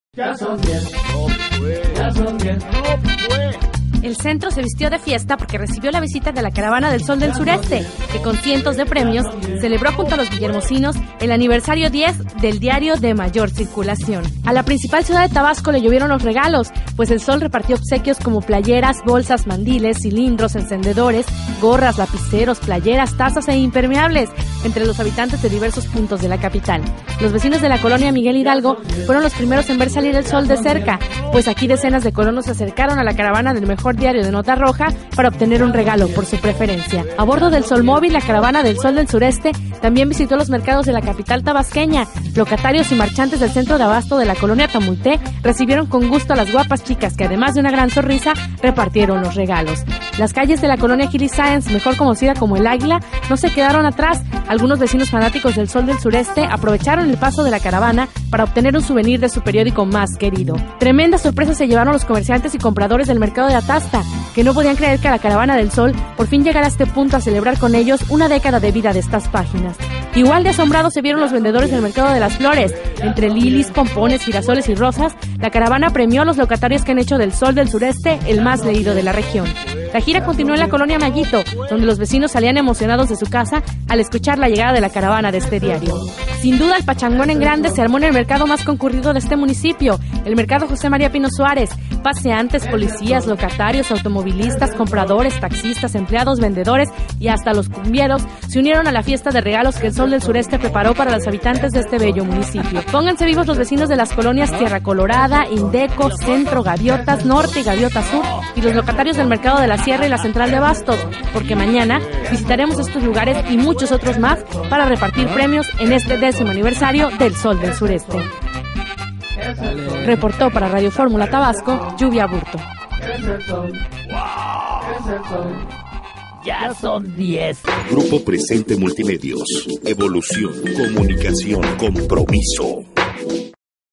Caravana El Sol del Sureste por Grupo Presente Multimedios
Reporte de la Caravana de El Sol del Sureste en 10 aniversario por Grupo Presente Multimedios.

Colonias de Centro
04/08/2011